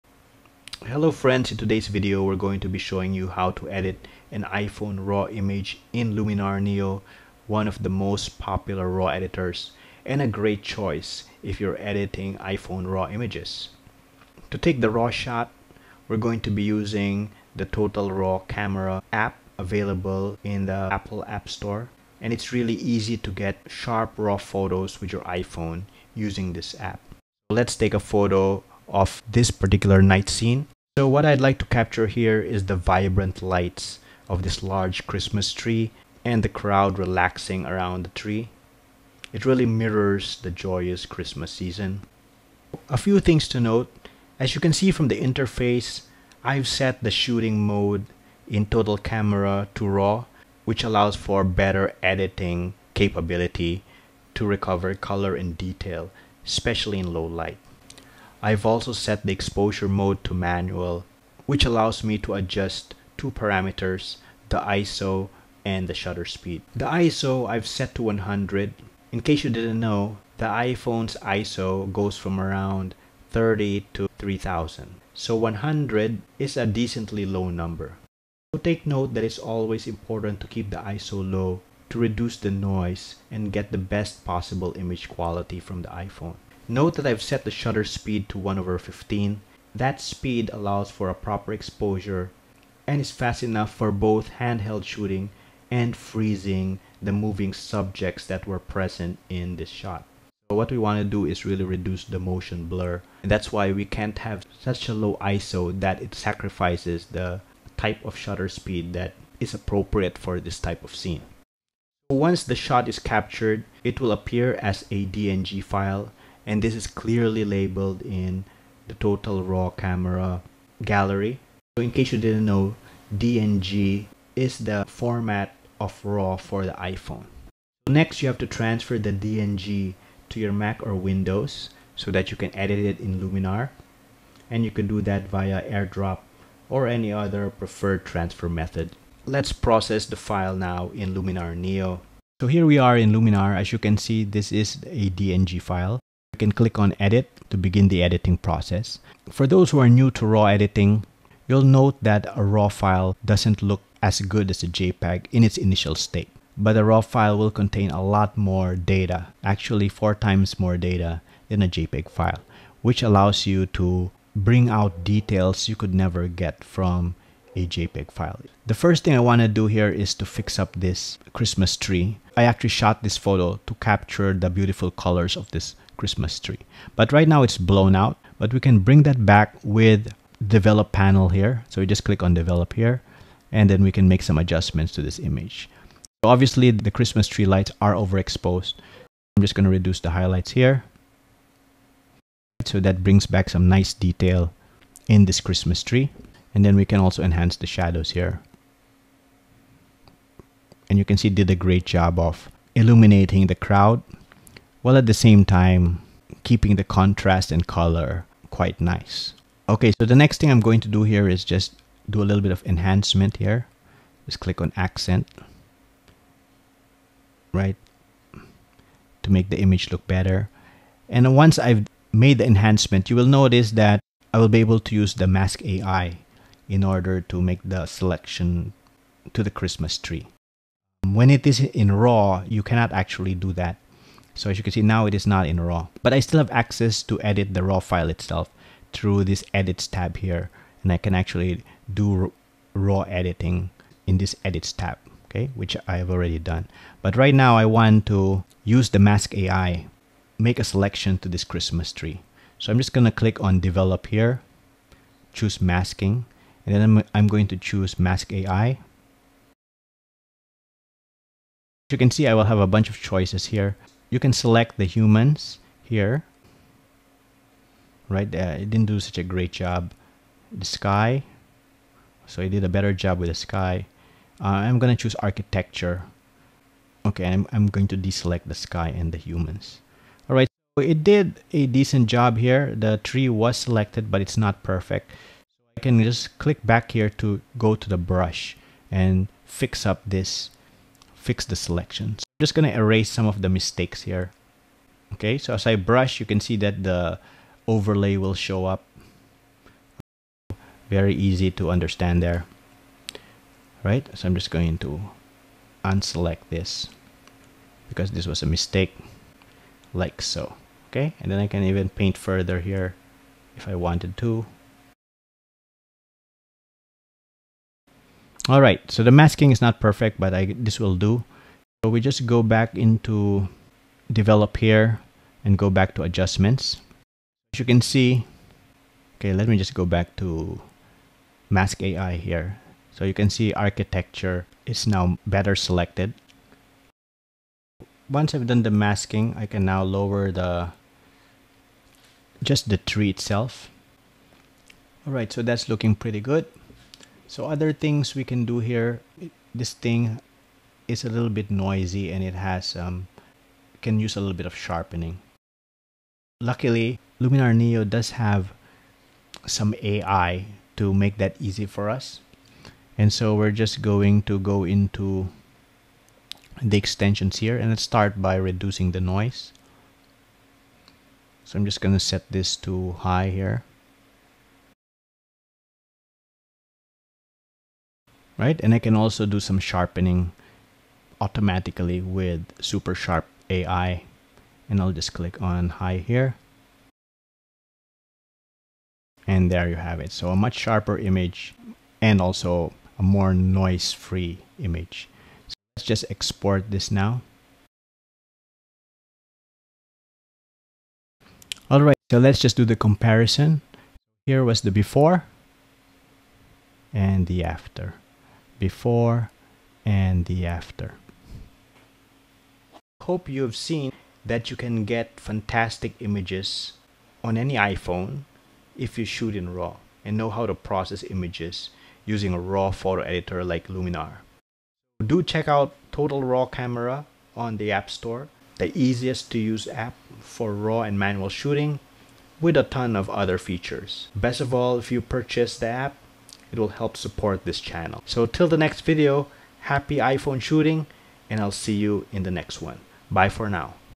iPHONE RAW SHOOTING: HOW TO PROCESS A RAW NIGHT IMAGE IN LUMINAR NEO
Want to get better images from your iPhone. Why not shoot in RAW? In this video we explain how to process a night image using the very capable Luminar Neo with extensions. Using the steps taught you can consistently shoot high quality night images and take advantage of Luminar Neo's power in delivering eye poppoing images.